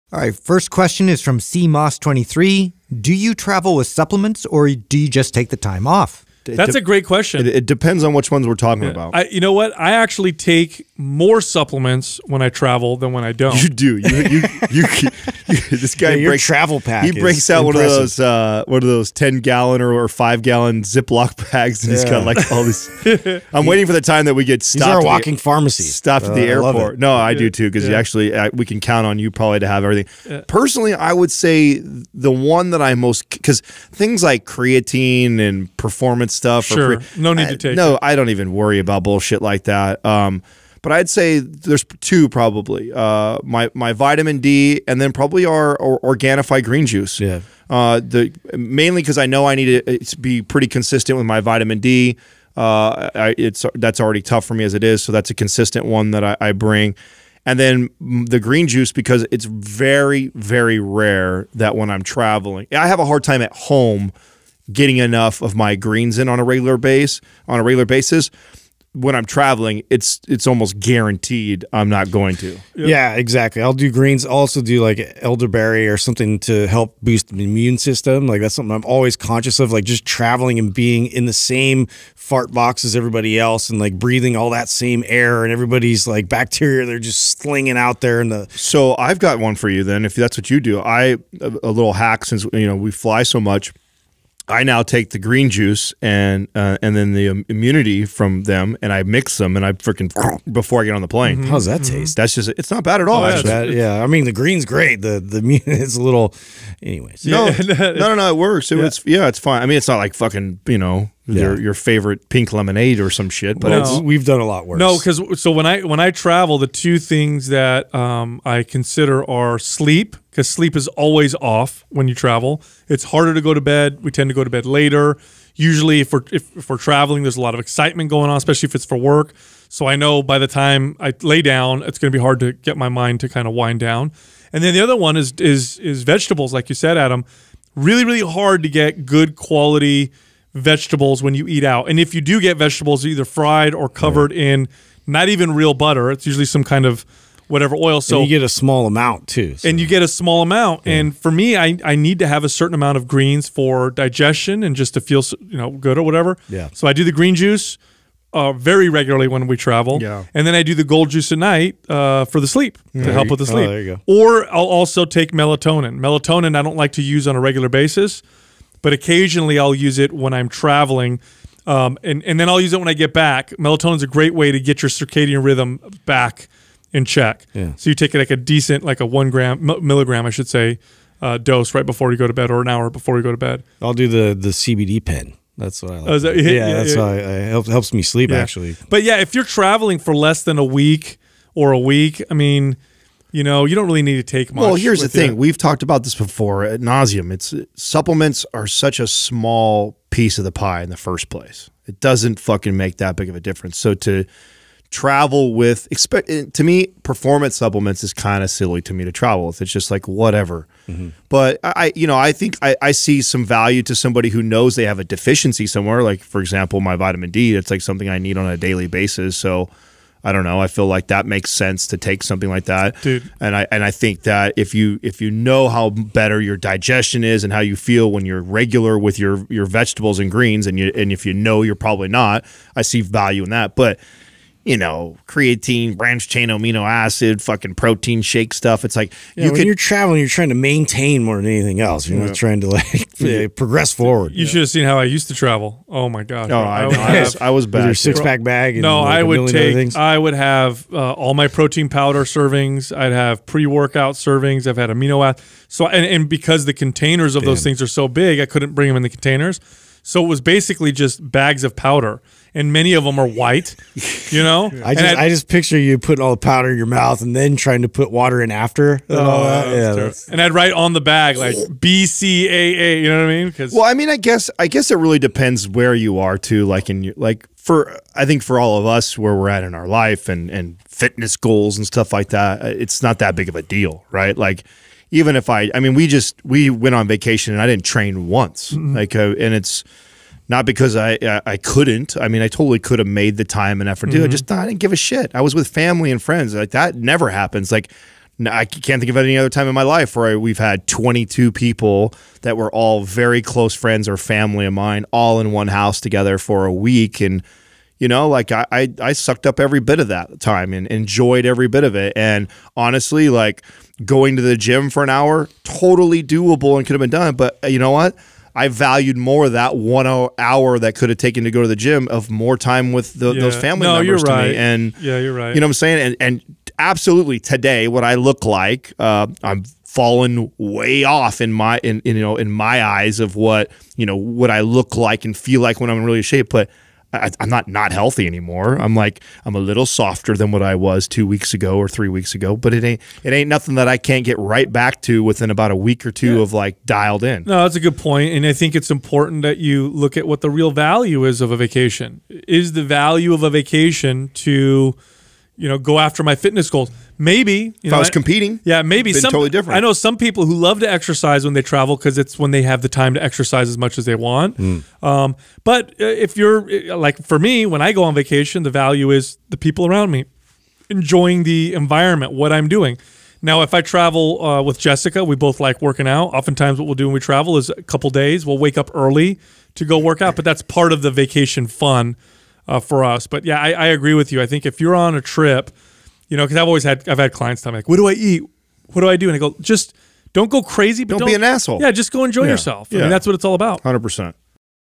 Tips For Traveling With Supplements
In this QUAH Sal, Adam, & Justin answer the question “Do you travel with supplements or do you just take the time off? ”